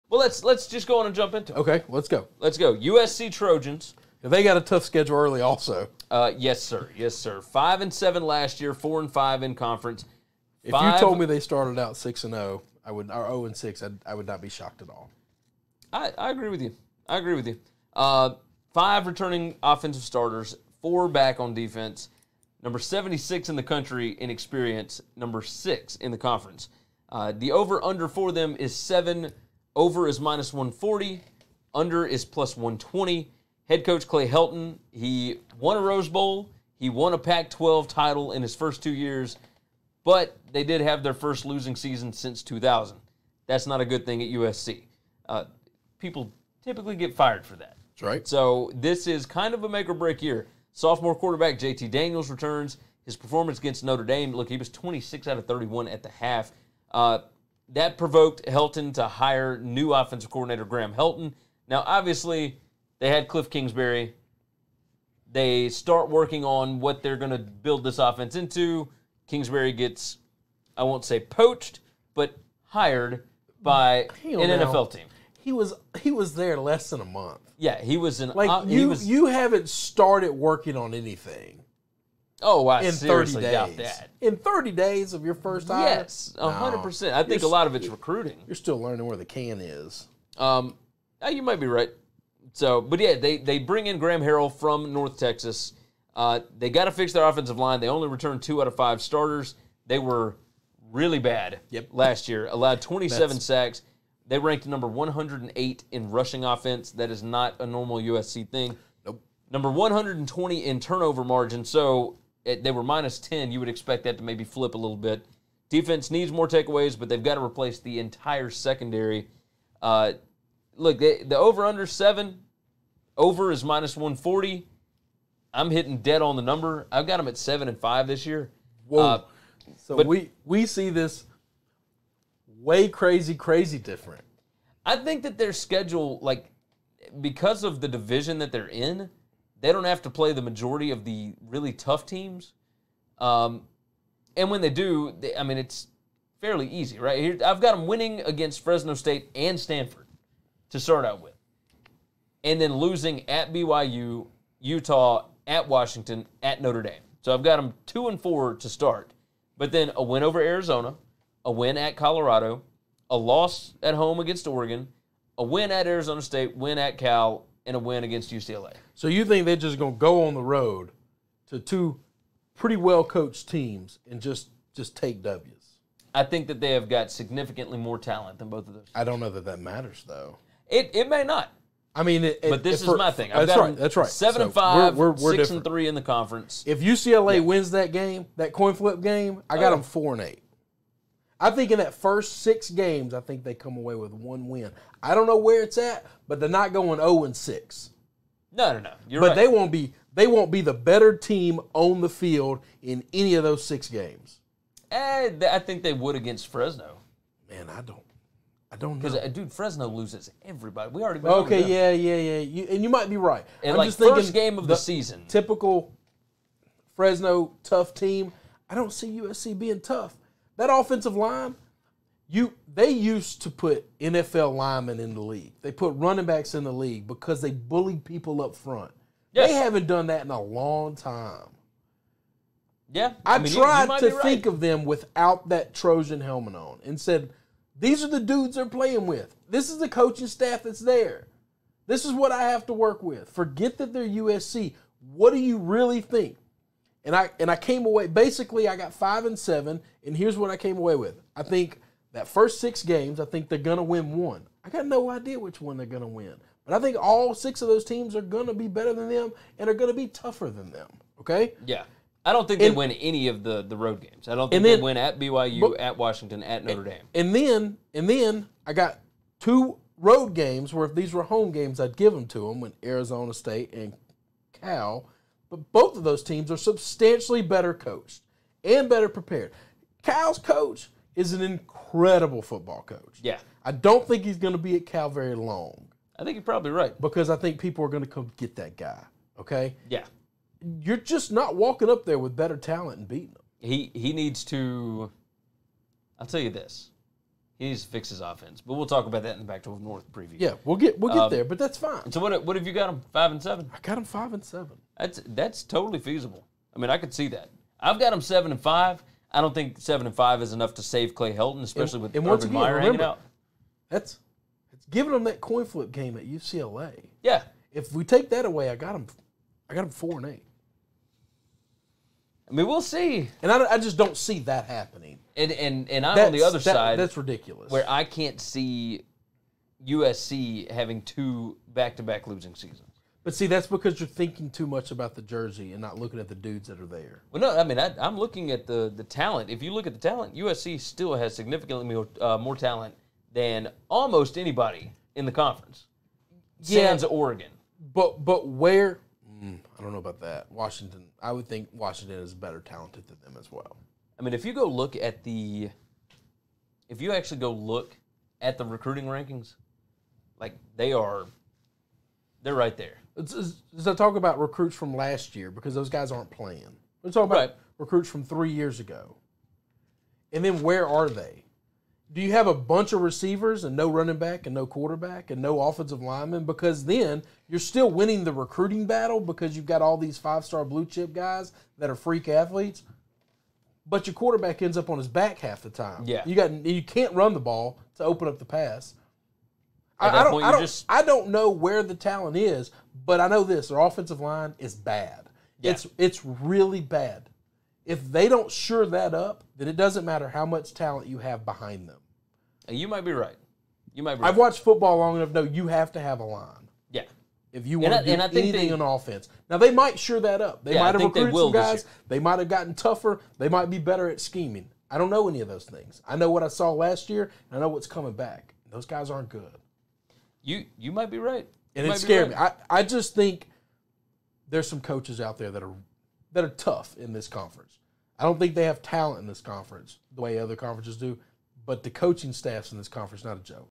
WCE: USC Trojans 2019 College Football Preview & Predictions
Our 2019 College Football preview and schedule record prediction for the USC Trojans from the PAC 12 South division.